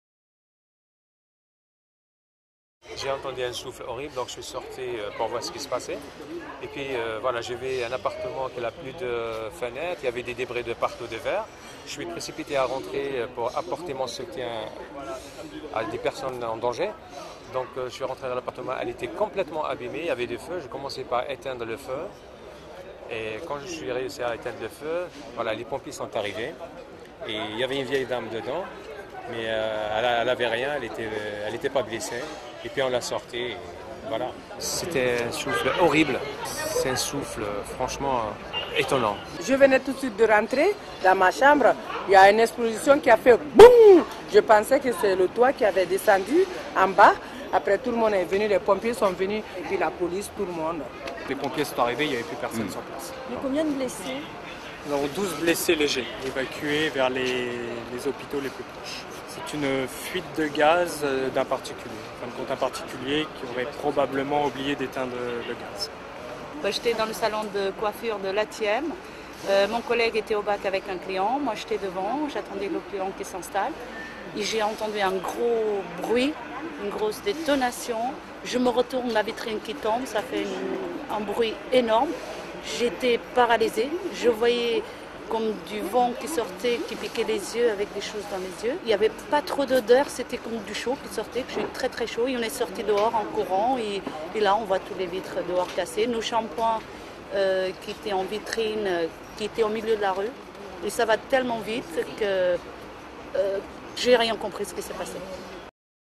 Explosion de gaz au 44bis rue de la Voûte dans le XIIe à Paris le 9 septembre 2008
Douze personnes ont été légèrement blessées après une explosion survenue au premier étage d'un immeuble situé au 44 bis rue de la Voûte à Paris (12e). Touchées pour la plupart par des morceaux de verre et des débris, les victimes ont toutes été transférées vers les hôpitaux les plus proches du secteur.
La déflagration s'est produite autour de 14 h 35 dans l'appartement d'une personne âgée de 79 ans, probablement sénile selon plusieurs témoignages. Par chance, le vieillard s'était absenté de son logement. Au total, une cinquantaine de personnes ont dû être évacuées du périmètre de sécurité installé après l'accident. A l'arrivée des pompiers, les résidents étaient déjà sortis de l'immeuble, dont le premier étage a été éventré.